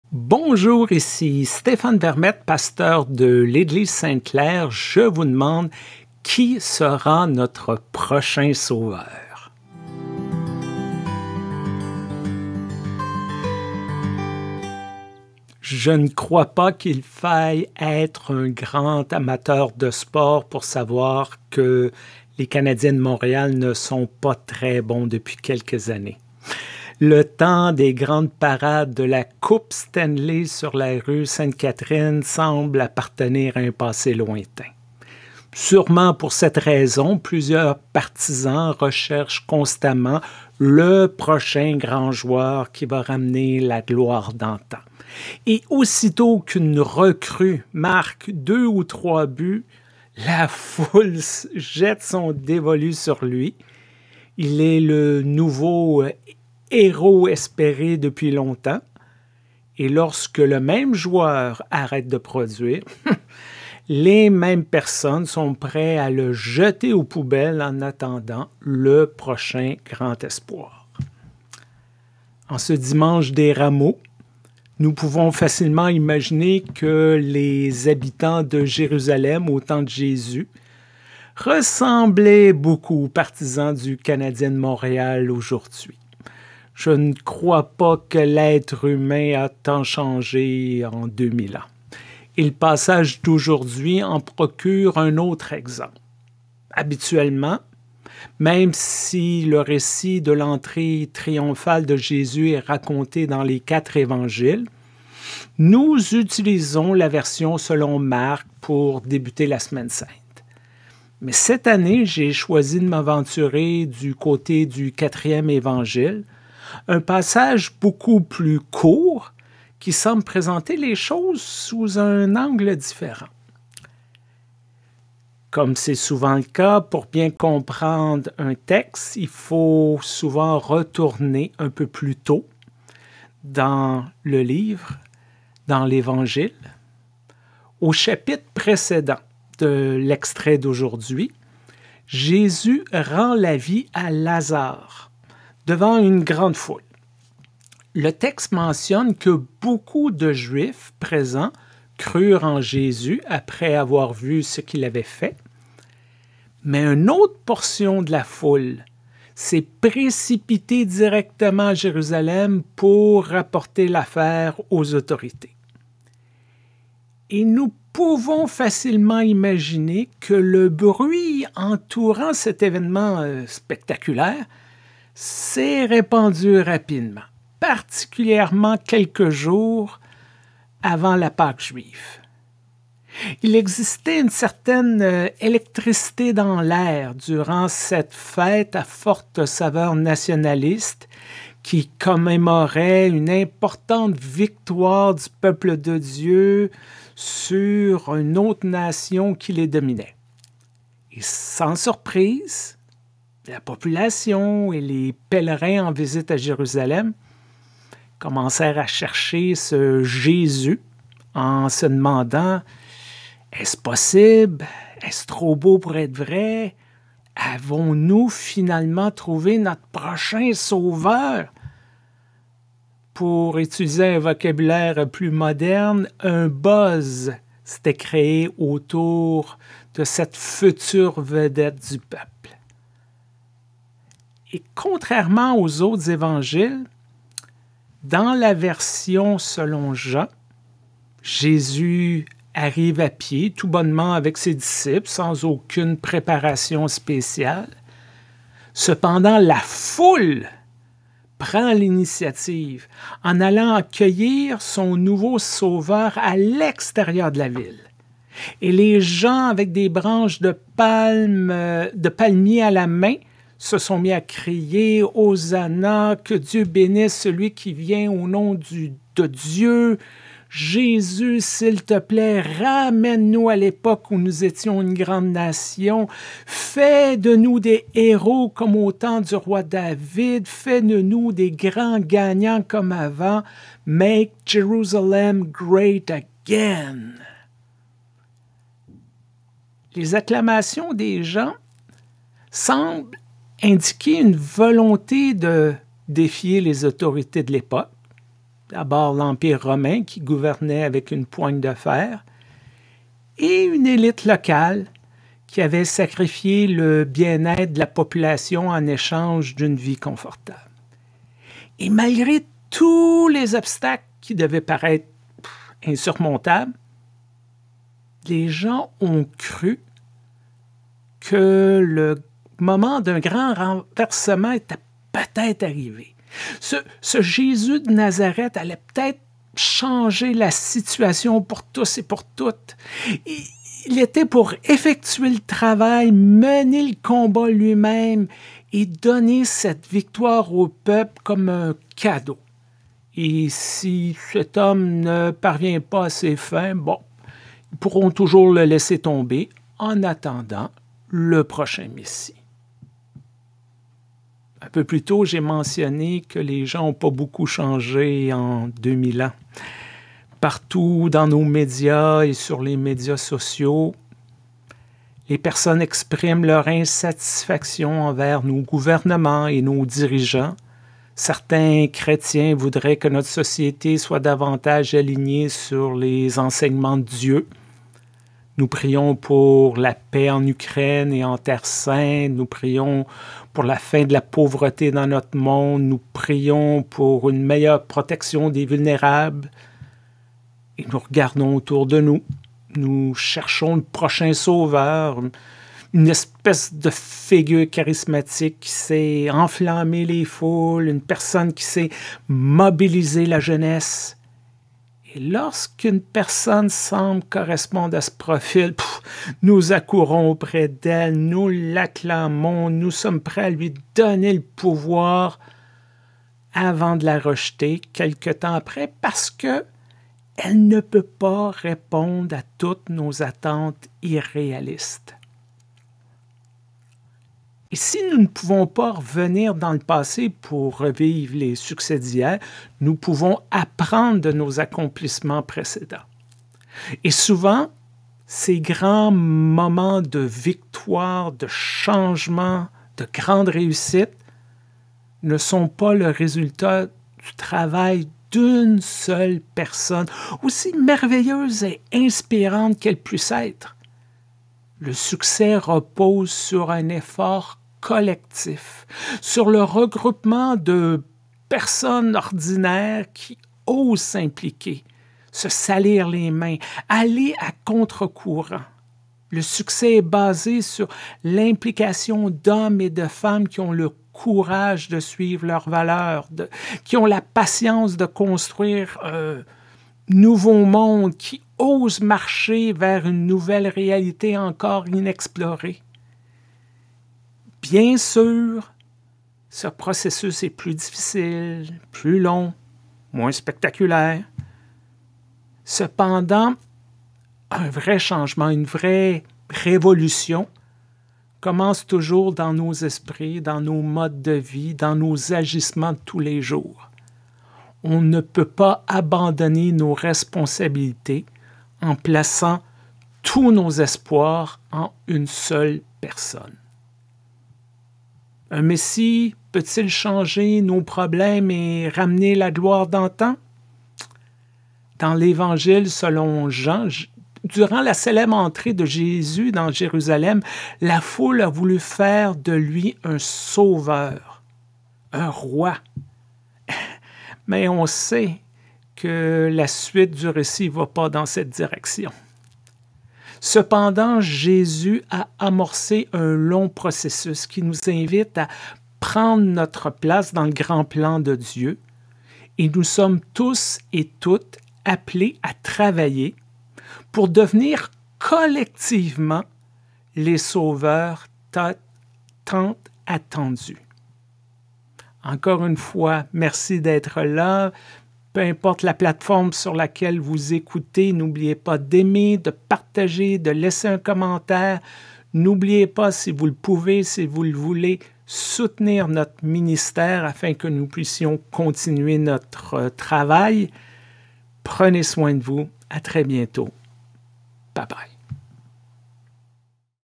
Qui sera notre prochain sauveur?
Même si le récit de l’entrée triomphale de Jésus est raconté dans les quatre évangiles, nous utilisons la version selon Marc pour débuter la Semaine sainte. Cette année, j’ai choisi de m’aventurer du côté du quatrième évangile; un passage beaucoup plus court qui semble présenter les choses sous un angle différent.

Au chapitre précédent l’extrait de ce soir, Jésus rend la vie à Lazare devant une grande foule. Le texte mentionne que beaucoup de Juifs présents crurent en Jésus après avoir vu ce qu’il avait fait. Mais une autre portion de la foule s’est précipitée directement à Jérusalem pour rapporter l’affaire aux autorités. Nous pouvons facilement imaginer que le bruit entourant cet événement spectaculaire s’est répandu rapidement, particulièrement à quelques jours de la Pâque juive. Il existait une certaine électricité dans l’air durant cette fête à forte saveur nationaliste. Sans surprise, la population ainsi que les pèlerins en visite à Jérusalem commencèrent à chercher Jésus en se demandant: Est-ce possible?  Est-ce trop beau pour être vrai?  Avons-nous finalement trouvé notre prochain sauveur?

Dans la version selon Jean Jésus arrive à pied tout bonnement avec ses disciples, sans aucune préparation spéciale. Cependant, la foule prend l’initiative en allant accueillir son nouveau sauveur à l’extérieur de la ville. Les gens, avec des branches de palmiers à la main, se sont mis à crier: Hosanna!  Que Dieu bénisse celui qui vient au nom de notre Dieu!  Jésus, s’il te plait, ramène-nous à l’époque où nous étions une grande nation! Fais de nous des gagnants comme avant!  Make Jerusalem Great Again!

Les acclamations des gens semblent indiquer une volonté de défier les autorités: l’Empire romain qui gouvernait la région avec une poigne de fer et une élite locale qui avait sacrifié le bien-être de la population en échange d’une vie confortable. Malgré tous les obstacles qui devaient paraître insurmontables, les gens ont cru que le moment d’un grand renversement était peut-être arrivé. Ce Jésus de Nazareth allait peut-être changer la situation pour tous et toutes. Il était pour effectuer le travail, mener le combat lui-même et donner cette victoire au peuple comme un cadeau. Et si cet homme ne parvient pas à ces fins, ils pourront toujours le laisser tomber en attendant le prochain Messie.

Un peu plus tôt, j’ai mentionné que les gens n’ont pas beaucoup changé en 2000 ans. Partout dans les médias et sur les médias sociaux, les personnes expriment leur insatisfaction envers nos gouvernements et nos dirigeants. Certains chrétiens voudraient que notre société soit davantage alignée sur les enseignements de Dieu. Nous prions pour la paix en Ukraine et en Terre sainte, la fin de la pauvreté dans notre monde et une meilleure protection des plus vulnérables. Nous regardons autour de nous et nous recherchons le prochain sauveur, une espèce de figure charismatique qui sait enflammer les foules. Et lorsqu’une personne semble correspondre à ce profil, nous accourons auprès d’elles, nous l’acclamons, nous lui sommes prêts à lui donner le pouvoir… avant de la rejeter quelque temps après parce qu’elle ne peut pas répondre à toutes nos attentes irréalistes.

Si nous ne pouvons pas revenir dans le passé pour revivre les succès d’hier, nous pouvons apprendre de nos accomplissements précédents. Souvent, ces grands moments ne sont pas le résultat du travail d’une seule personne, aussi merveilleuse et inspirante qu’elle puisse être. Le succès repose sur un effort collectif, sur le regroupement de personnes ordinaires qui osent s’impliquer, se salir les mains et aller à contre-courant. Le succès est basé sur l’implication d’hommes et de femmes qui ont le courage de suivre leurs valeurs, qui ont la patience de construire un nouveau modèle, qui osent marcher vers une nouvelle réalité encore inexplorée. Bien sûr, ce processus est plus difficile, plus long et moins spectaculaire. Cependant, une vraie révolution commence toujours dans nos esprits, nos modes de vie, nos agissements tous les jours. Nous ne pouvons pas abandonner nos responsabilités en plaçant tous nos espoirs en une seule personne.

Un messie peut-il régler tous nos problèmes et ramener la gloire d’antan? Dans l’évangile selon Jean, durant la célèbre entrée de Jésus dans Jérusalem, la foule a voulu faire de lui un sauveur, mais nous savons que la suite du récit ne va pas dans ce sens. Cependant, Jésus a amorcé un long processus qui nous invite à prendre notre place dans le grand plan de Dieu.  Nous sommes tous et toutes appelés à travailler pour devenir collectivement les sauveurs tant attendus. Amen.